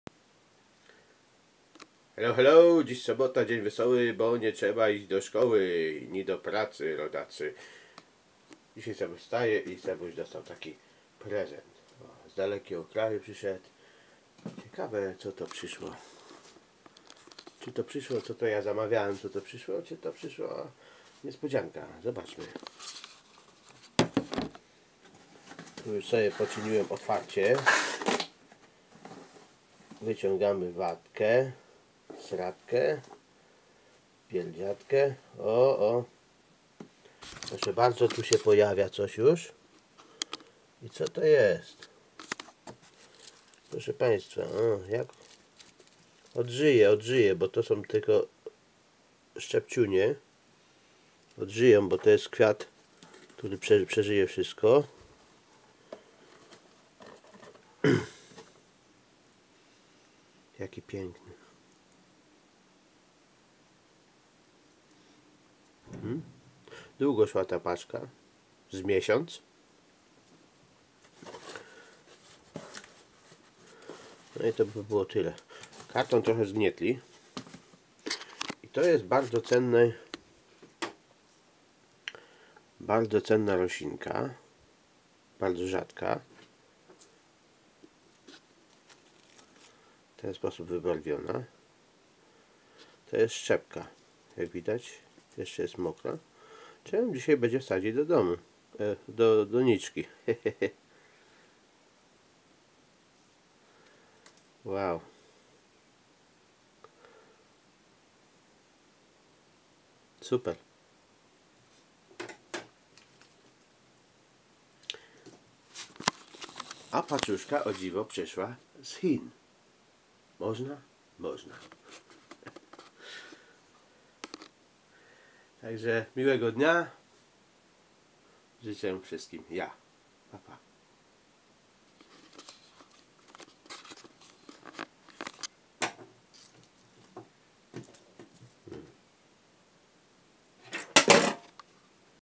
Paczka z chin - Aeonium Medusa
Kto w sobotę rankiem późnym wstaje ten wymarzone prezenty dostaje ❤🤞😁 Właśnie przyszły szczepki bardzo rzadkiego i cennego AEONIUM MEDUSA ❤🤞 czyniąc mój dzień szczęśliwym wielce 🙂 plus extra gift sempervivum